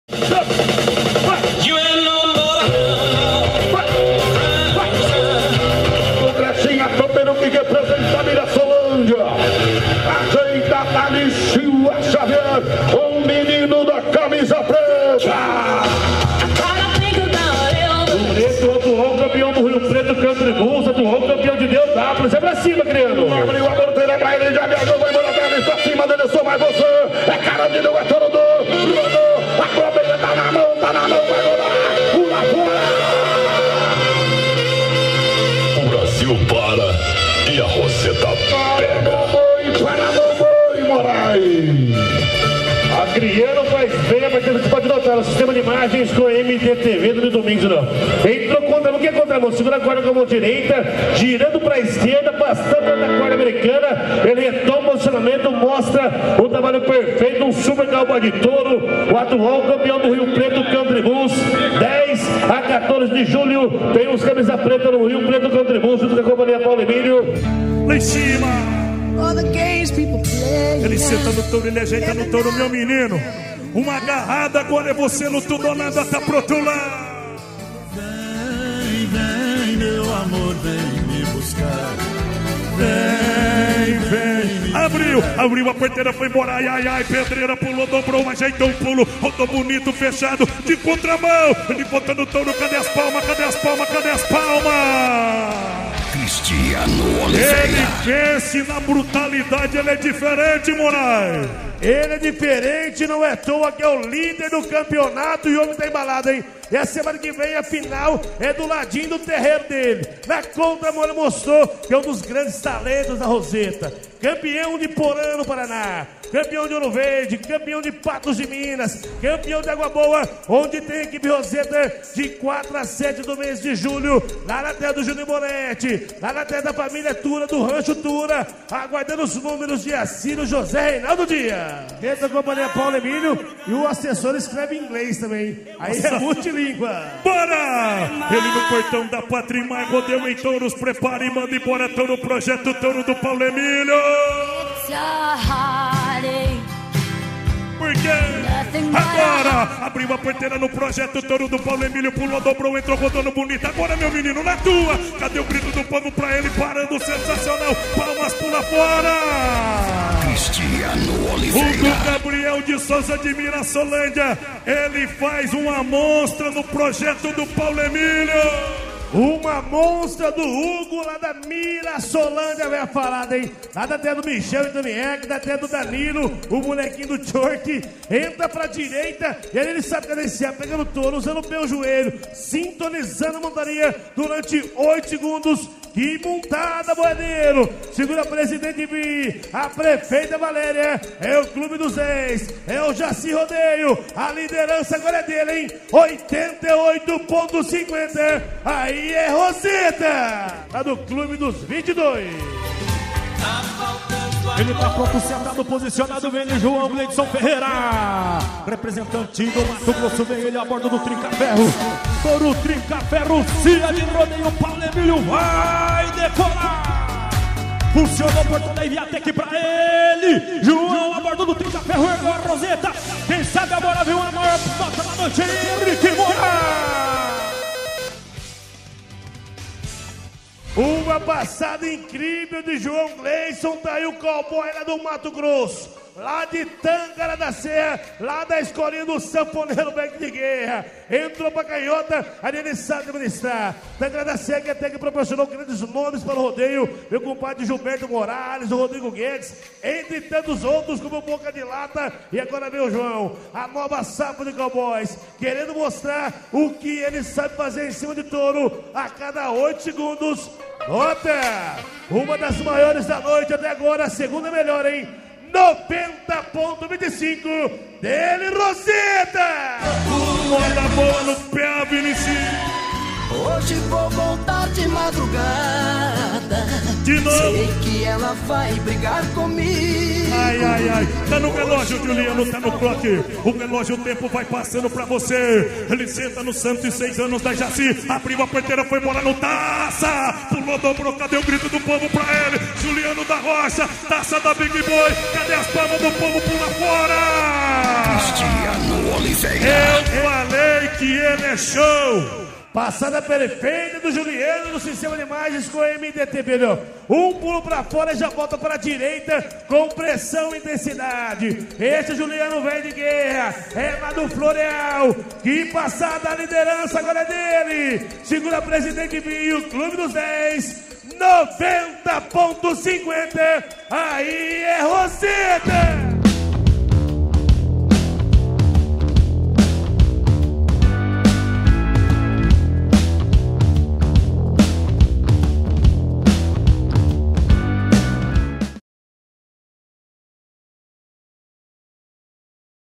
JACI-SP 2024 | QUINTA / MELHORES MONTARIAS EM TOUROS
RODEIO DE JACI-SP 2024
83ª ETAPA EKIP ROZETA TEMPORADA 2023/2024

QUINTA / MELHORES MONTARIAS EM TOUROS

00:08 • Tales Silva Xavier de Feijó - AC vs Carandirú - Cia. J.H / Crechinha | Nota: 87,25
01:08 • Assyrio José Reinaldo de Sertanópolis - PR vs Multiplicação - Cia. Big Boi / Rodrigo Maia | Nota: 87,50
02:13 • Hugo Gabriel de Souza de Mirassolândia - SP vs Projeto - Cia. Paulo Emílio | Nota: 88,50
03:21 • João Gleyson Ferreira de Tangará da Serra - MT vs Trinca Ferro - Cia. Paulo Emílio | Nota: 90,25
04:30 • Juliano da Rocha de Floreal - SP vs Taça - Cia. Big Boi / Rodrigo Maia | Nota: 90,50

Edição e Imagens: Guilherme Ferreira